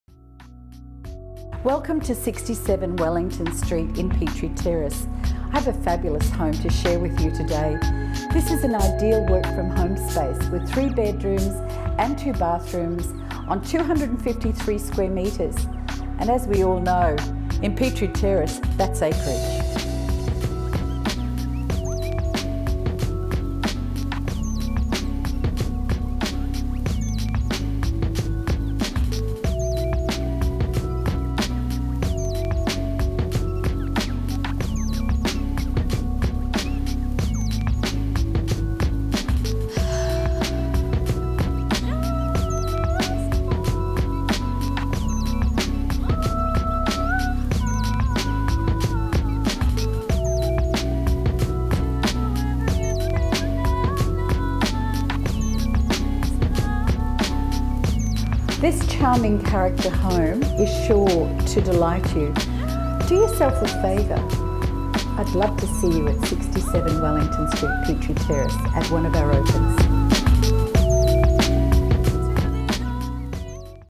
SOLD 67 Wellington Street Petrie Terrace - Glynis Austin
Positioned in the highly desirable 4000 postcode, this 3 bedroom 2 bathroom home possesses the much-coveted layout of open plan living, dining and kitchen straight out to a sparkling pool. Picture yourself being able to easily walk home from work while your children are able to walk to and from Brisbane Grammar Schools.

This home has character and charm aplenty and updated for modern living representing much of what is so loved about Petrie Terrace and its historical enclave.  Come and take a look to see for yourself!

Features:
· Gorgeous street appeal
· Master bedroom with en-suite
· Huge dedicated study straight off the front foyer providing the perfect work from home office
· Open plan living, dining and kitchen opening out to pool for ideal entertaining
· All bedrooms with built-in robes
· Sparkling in-ground pool with paved patio for easy entertaining and direct access from the street
· Character features throughout incl. bay windows, feature brick fireplace, deck/patios and polished timber floors
· Secure off street parking for two cars (tandem)
· Air-conditioned
· Ample storage
· Petrie Terrace SS catchment area
· Kelvin Grove SC catchment area
· Walking distance to Brisbane Grammar Schools and Terrace
· Less than a kilometre to Brisbane's CBD

This is an exceptional location, can you imagine walking to the Barracks to dine and see a movie?  Walk to the CBD?  Walk to Brisbane's Cultural Centre?  Walk to Suncorp Stadium?  So easy!

This address further provides easy access to the ICB for those who travel regularly, both North and South coastal roads and the Western Freeway.  If you are looking to make a lifestyle change and see yourself home from work in minutes, this is your new home!

Auction: 30th April 2019 - 6:30PM, 157 Given Terrace, Paddington